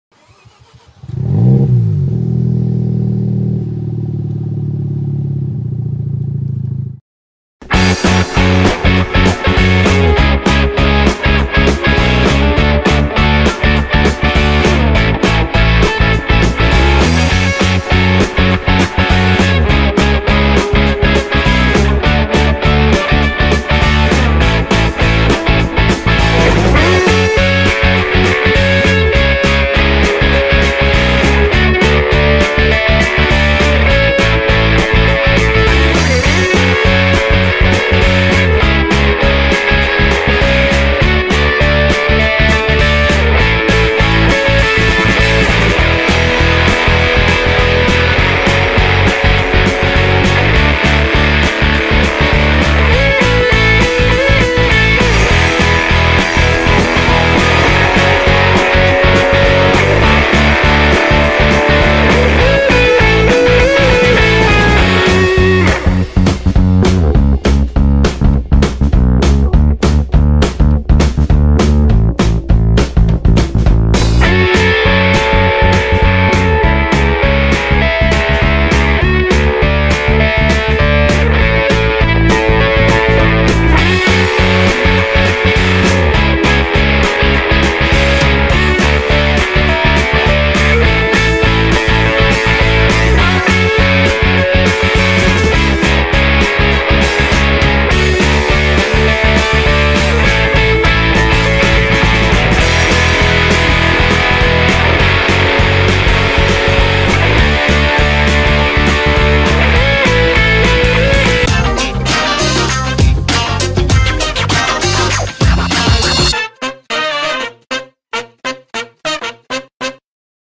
Oh my Lagonda
What's big, British and currently sits in an auction yard? An Aston Martin Lagonda!